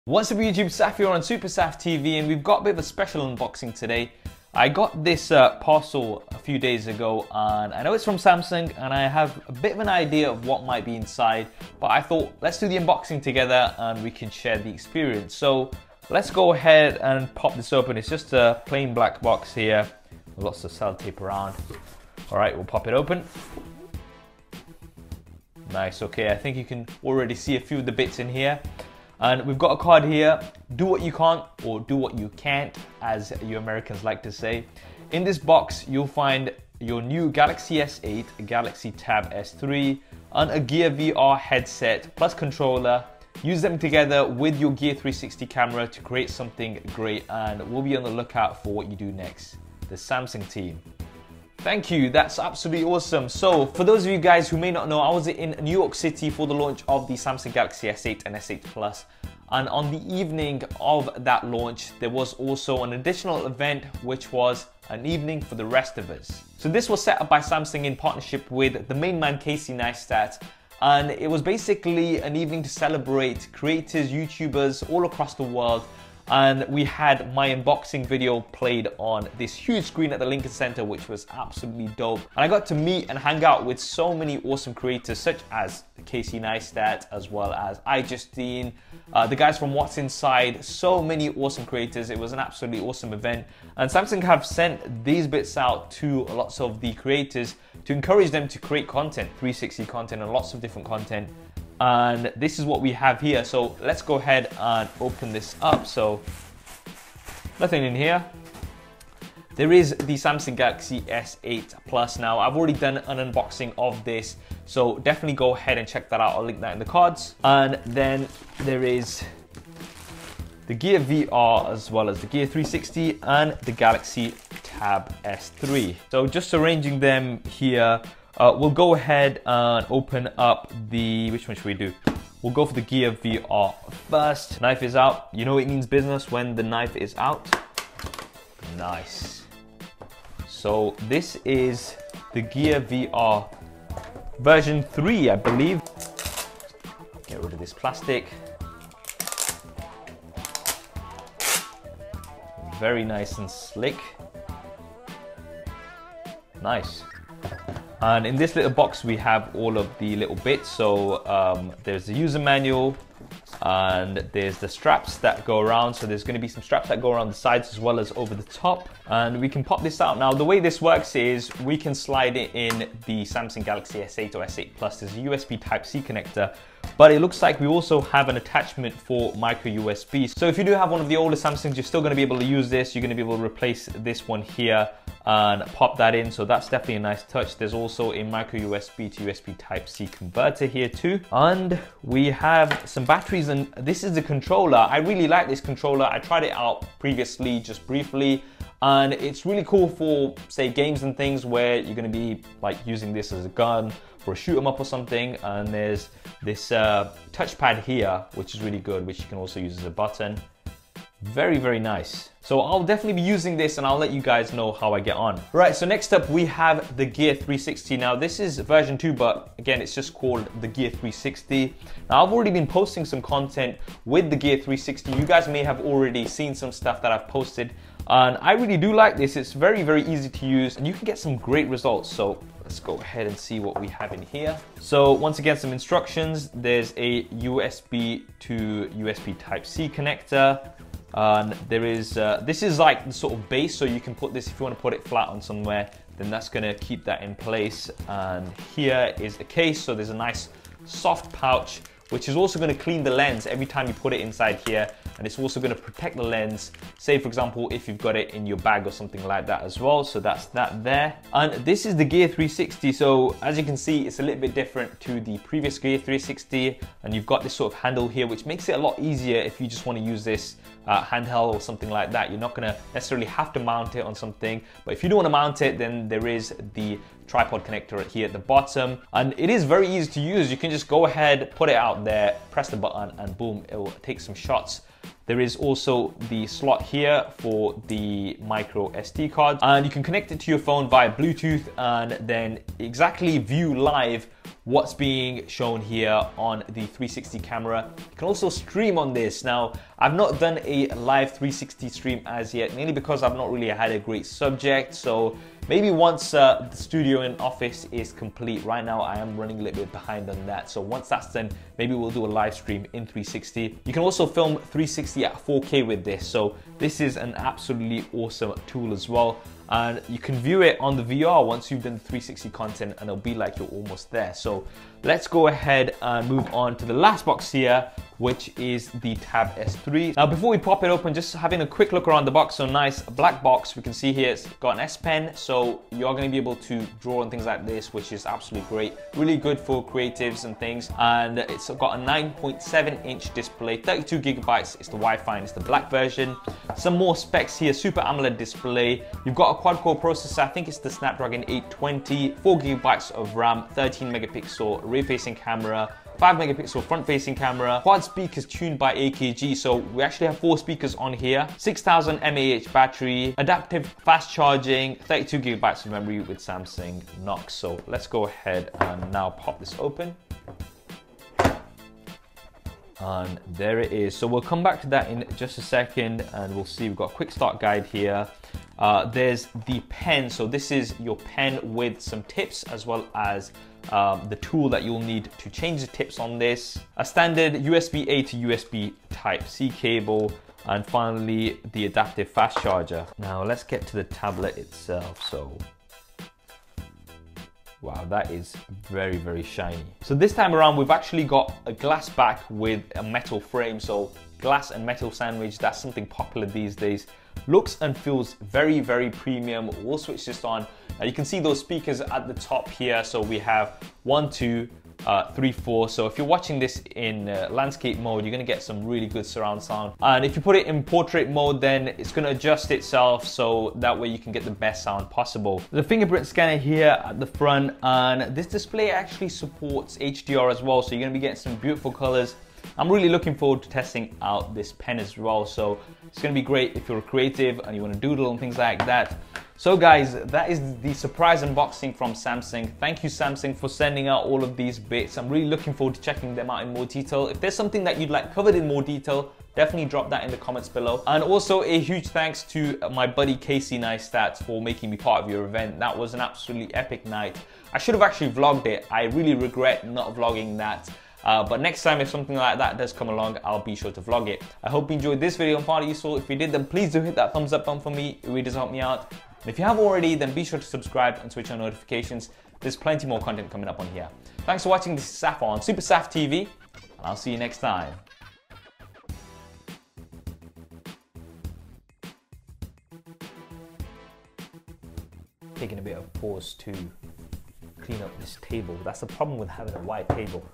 Mystery package from SAMSUNG - Do What You Can't
Samsung sent me a surprise package following the Casey Neistat Do What You Can't Event.

Links to featured products:

US UK

Samsung Gear VR + Controller
US UK

Samsung 360 Gear 2017
US UK

US UK